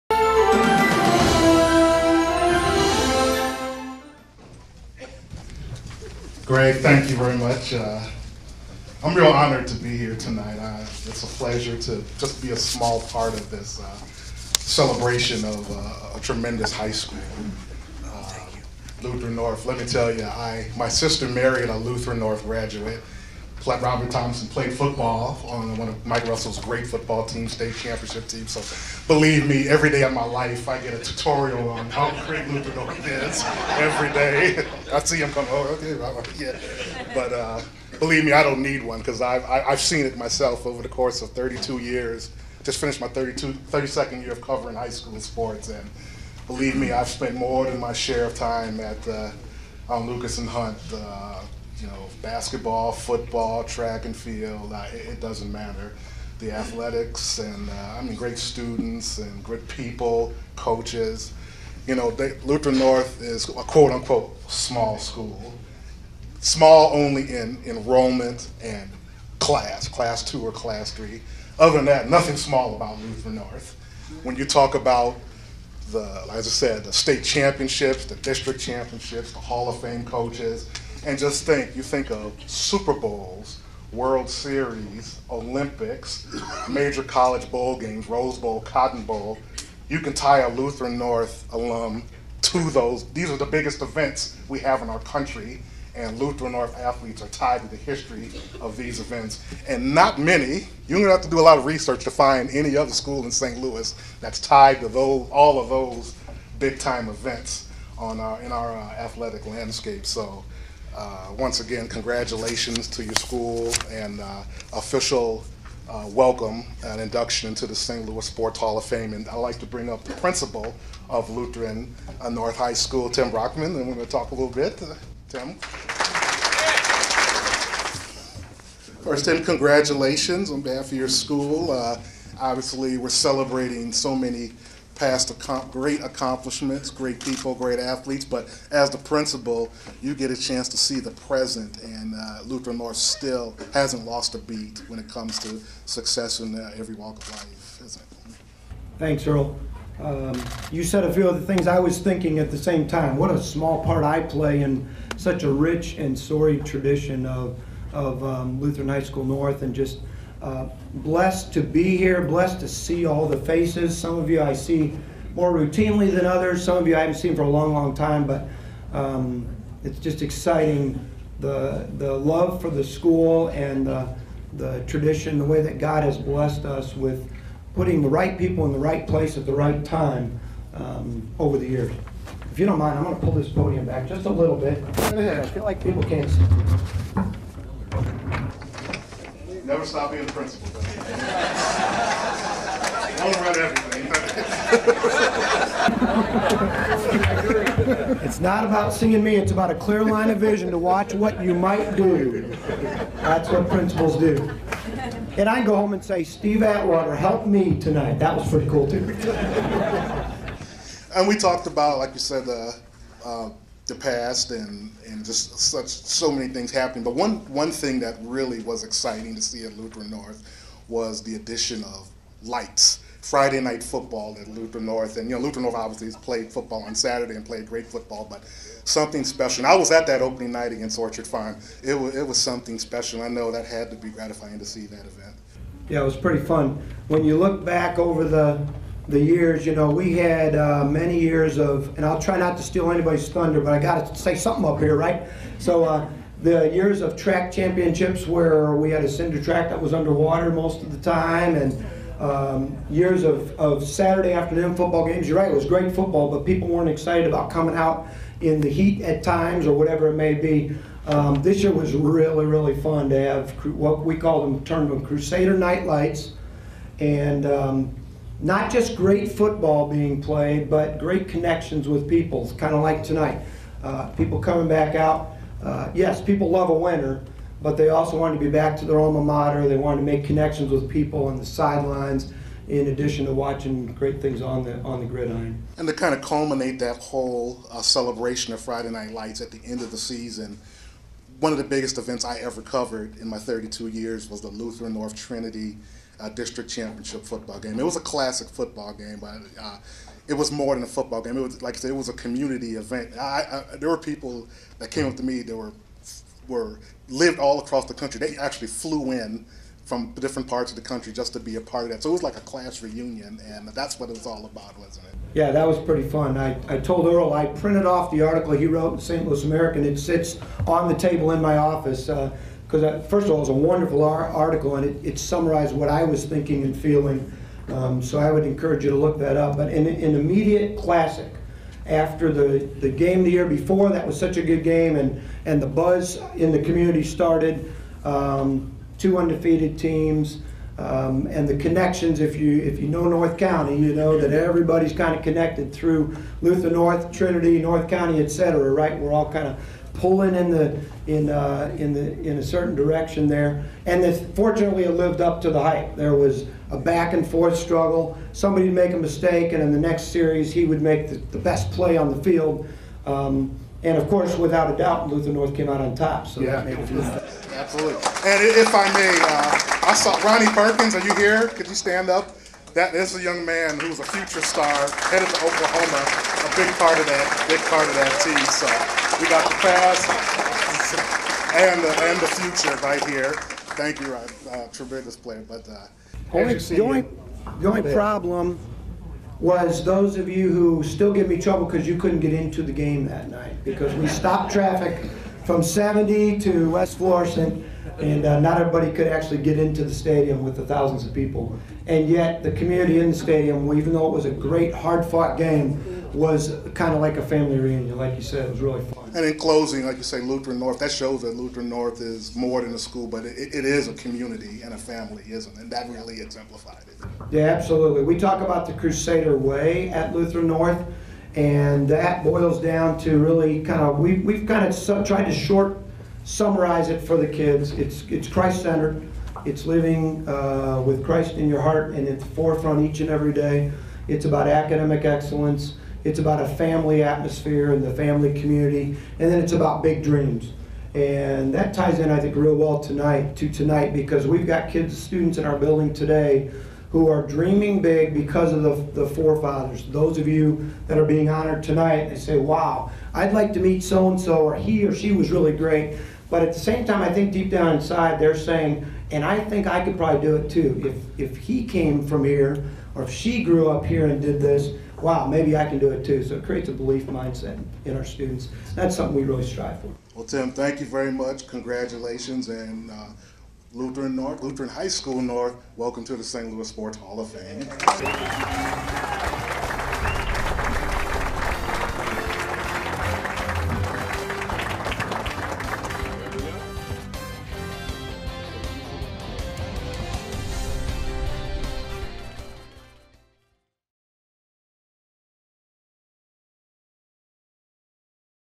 Tim Brackman
Dr. Tim Brackman, Principal of Lutheran North High School, is interviewed by Earl Austin Jr as he accepts the induction of the school into the St. Louis Sports Hall of Fame on May 31, 2018.